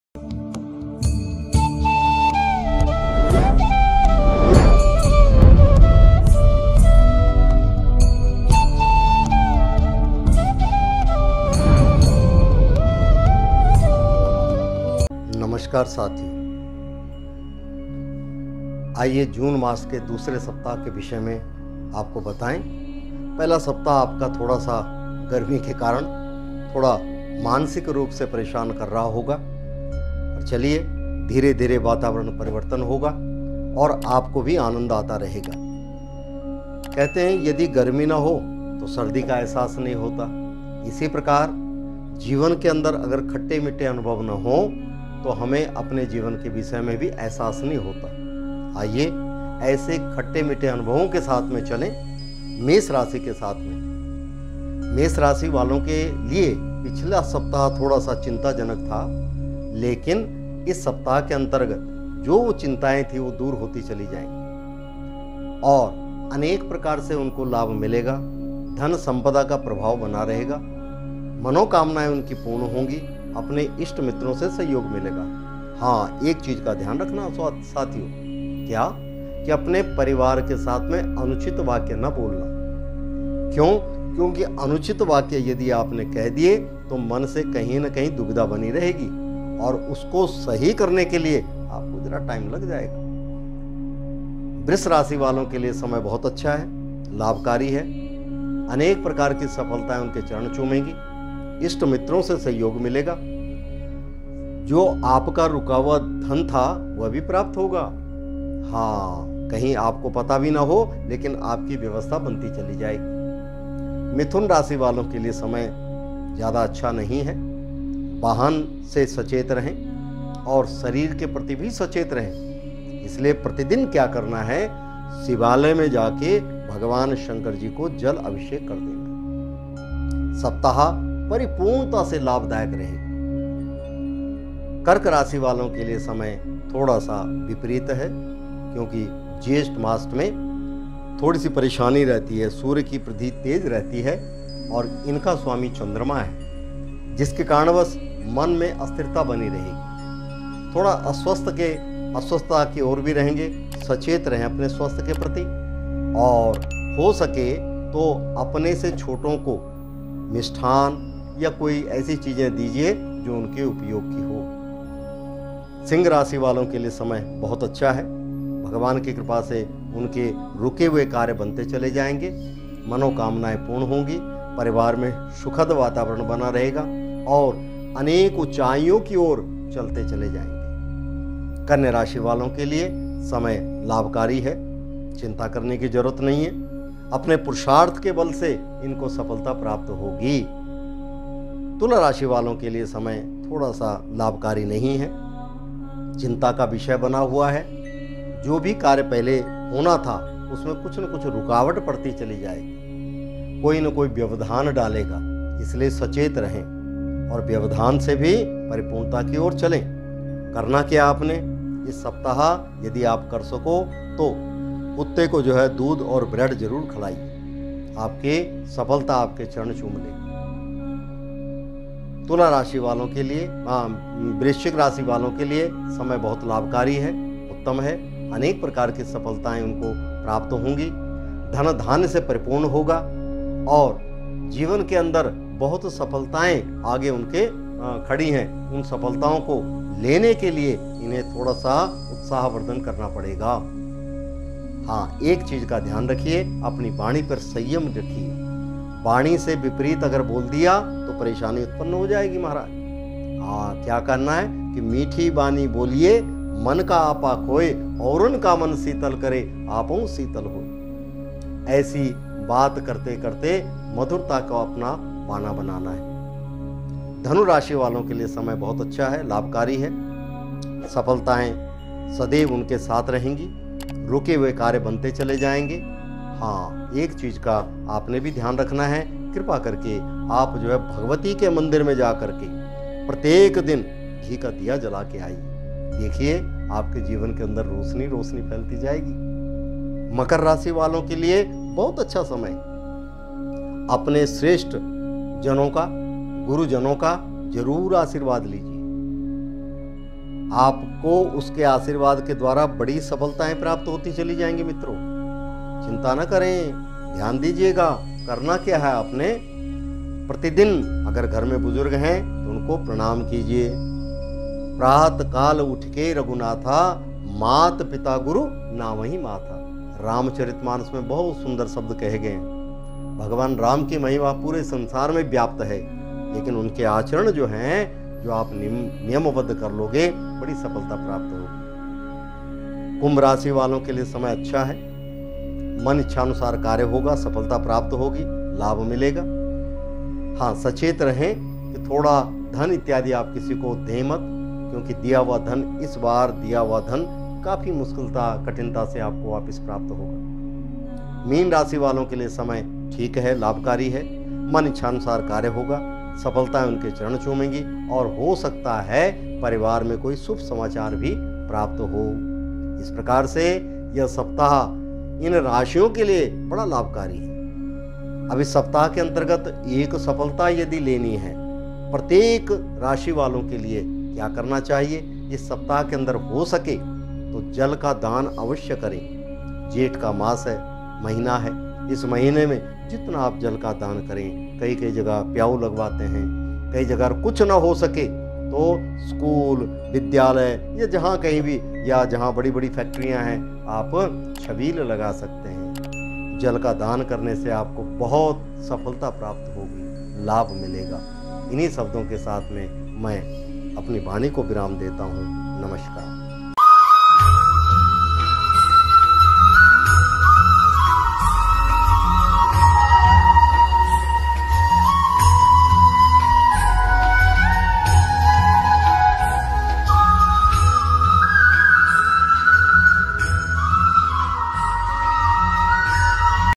Weekly Horoscope - 6 June to 12 June | साप्ताहिक राशिफल 6 जून से 12 जून
Janiye iss saptah 6 June to 12 June tak ka saptahik rashifal.

जाने इस सप्ताह 6 जून से 12 जून तक का सप्ताहिक राशिफल।

Images by